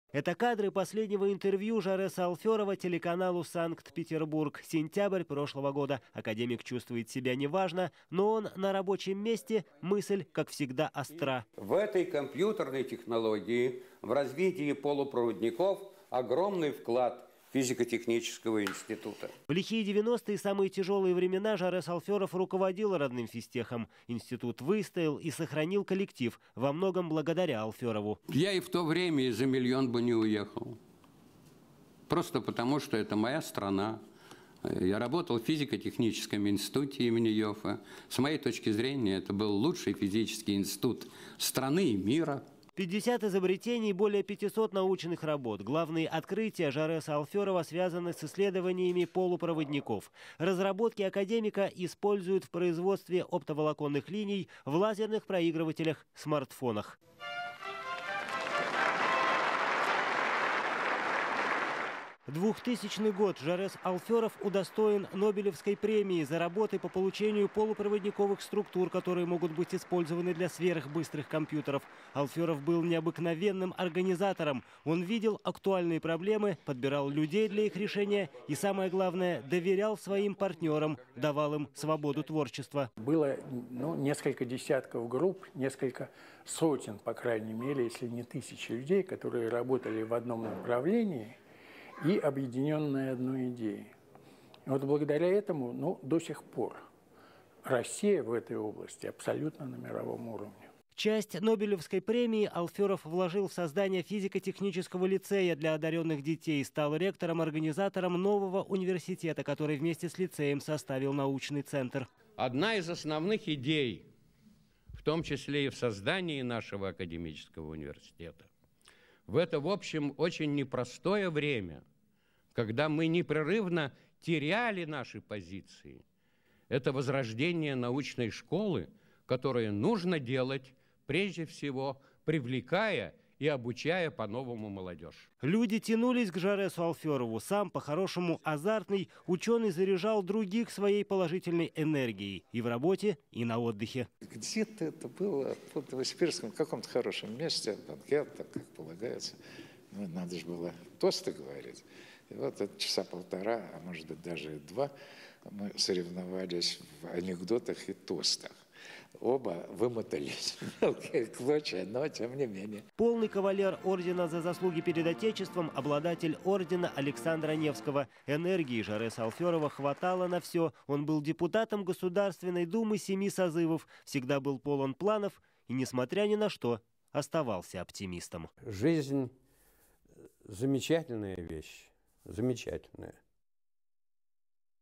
«Жизнь — замечательная вещь, замечательная». В память о Жоресе Алферове
Жорес Алферов будет погребён на кладбище в Комарово под Петербургом. В городе создана комиссия по организации похорон. Возглавит ее лично Александр Беглов. Ранее градоначальник направил родным и близким ученого слова поддержки. Выразил соболезнования и спикер ЗакСа Вячеслав Макаров. О гении времени скорбит весь мир.